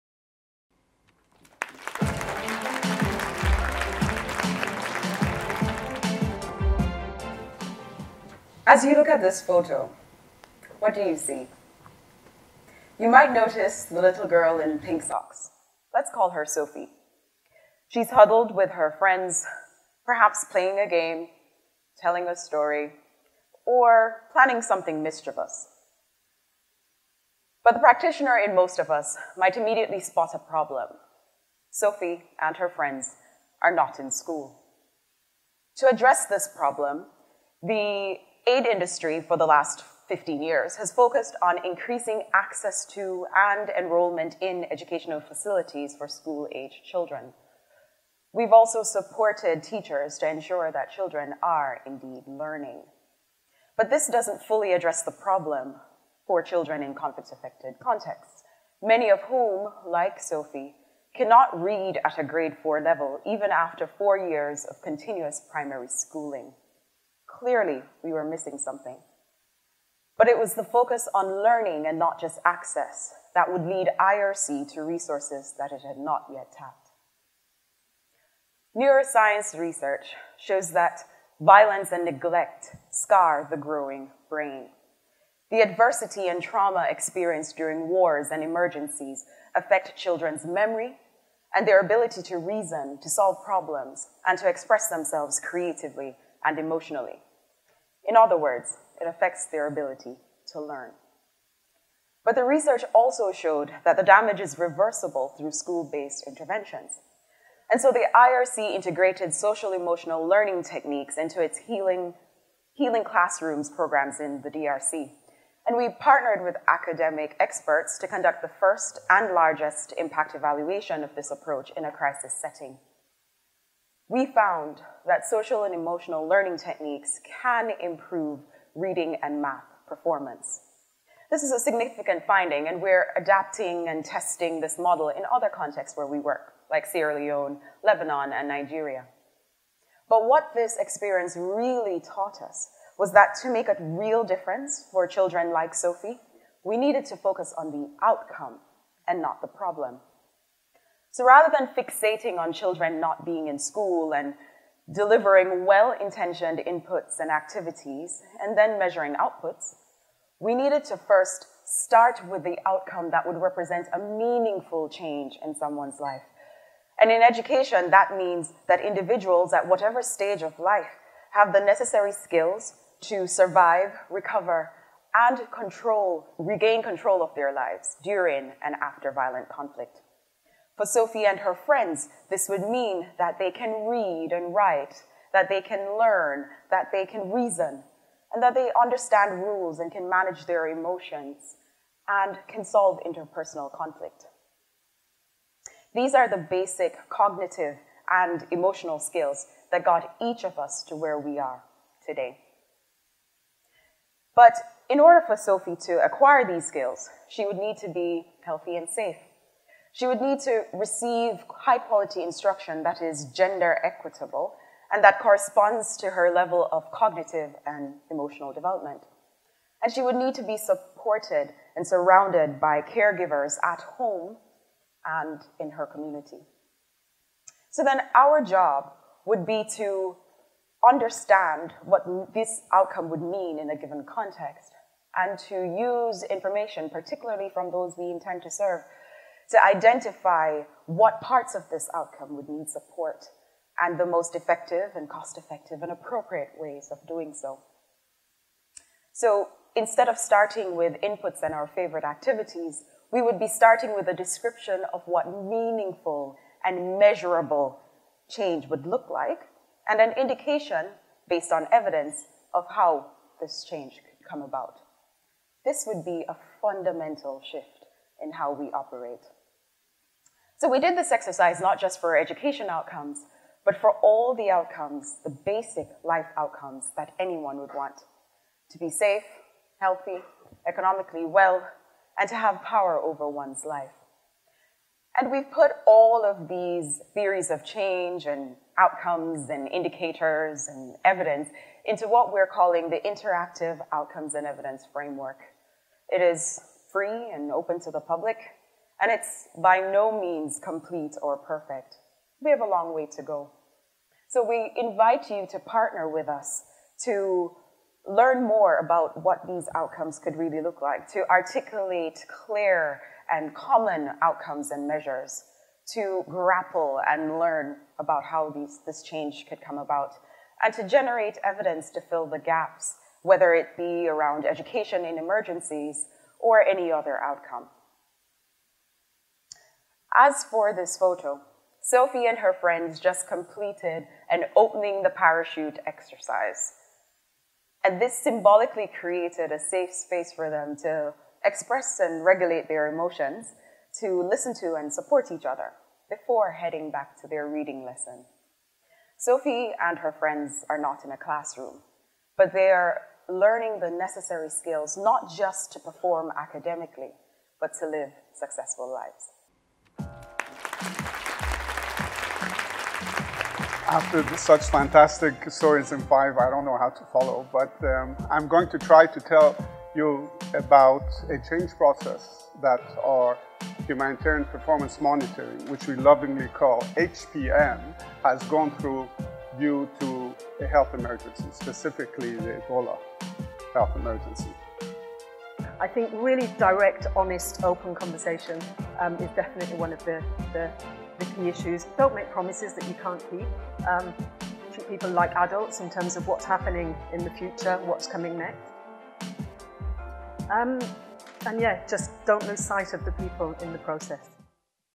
Story in 5: Sheree Bennett, Outcomes and Evidence Framework
Recorded at 'Changing Humanitarian Action?', ALNAP's 31st Annual Meeting.